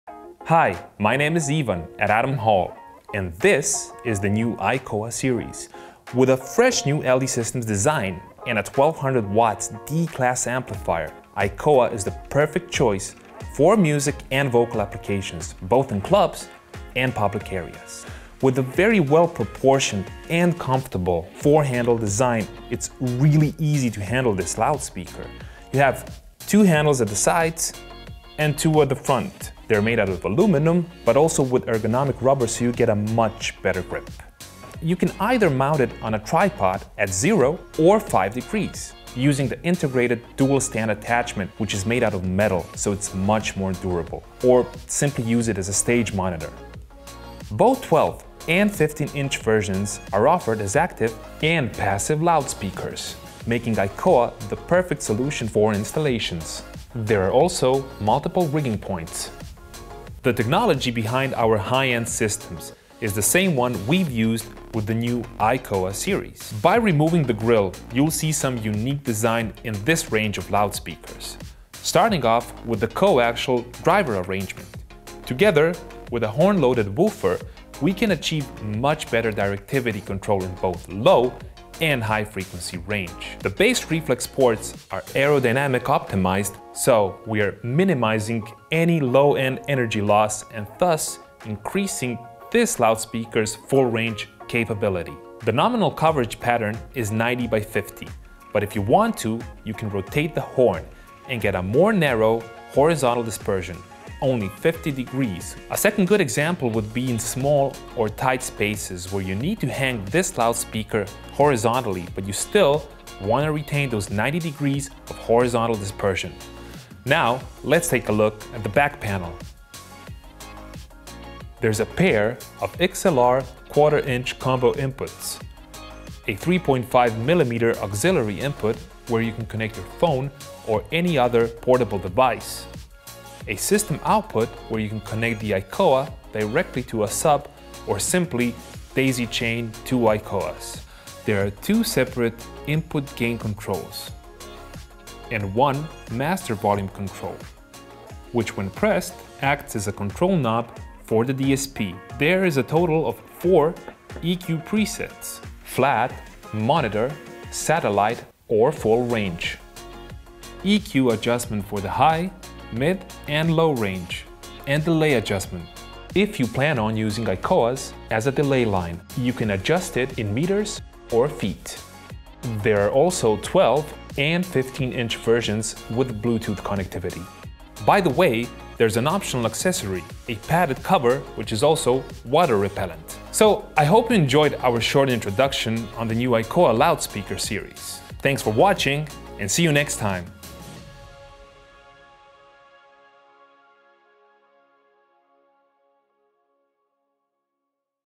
LD Systems ICOA Series - Powered Coaxial Full-Range Loudspeaker
Discover the new ICOA® Series by LD Systems® with a coaxial driver arrangement and rotatable horn.

LD Systems® is a brand of the Adam Hall Group.